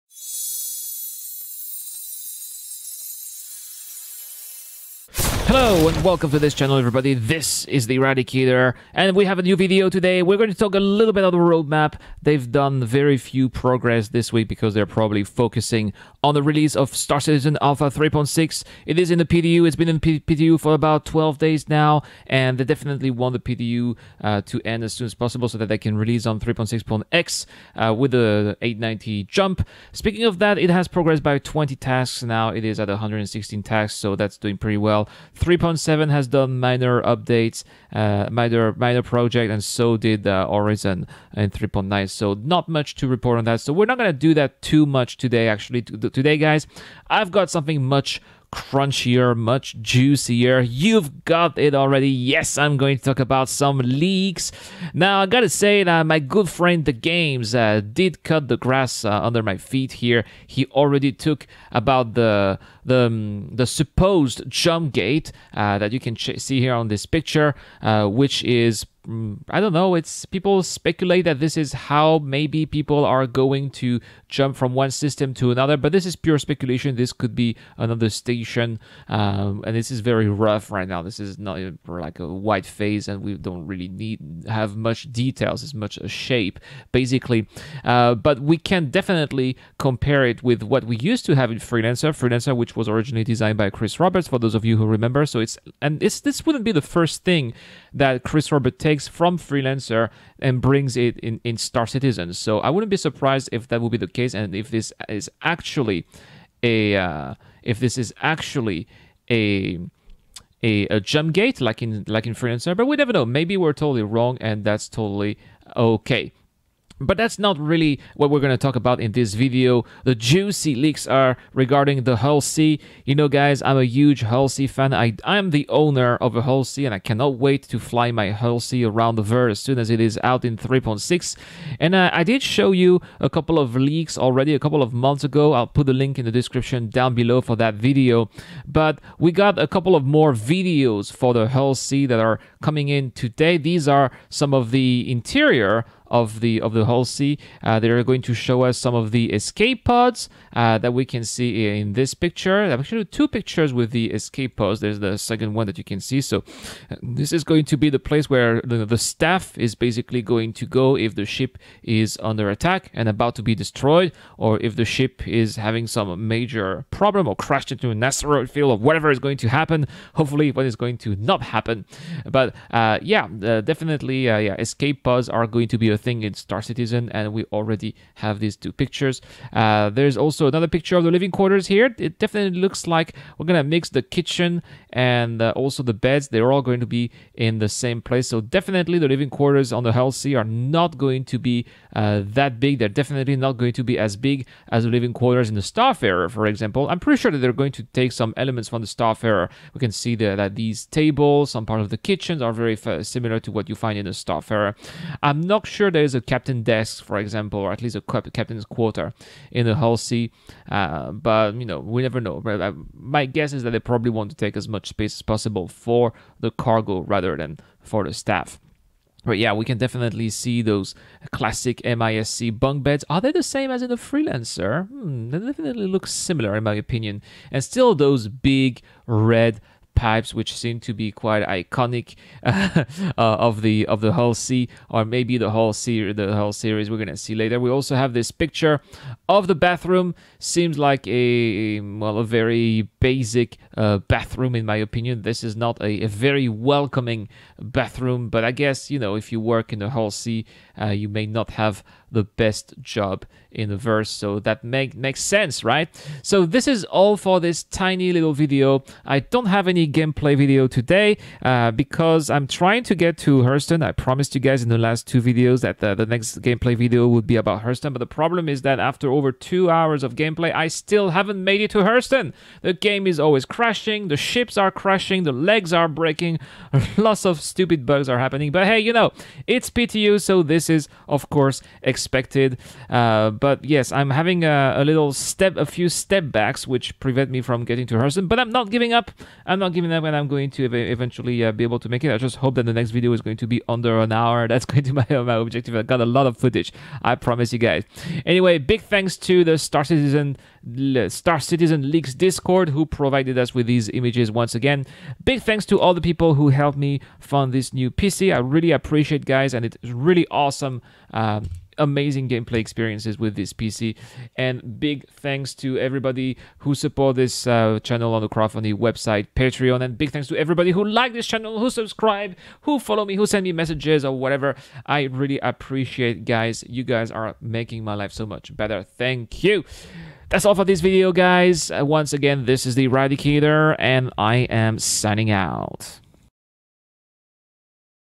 Star Citizen 3.8 Leaks - More Hull C Pictures
More leaks regarding the hullC are out, thanks to the Star Citizen leaks discord. Let's check them out.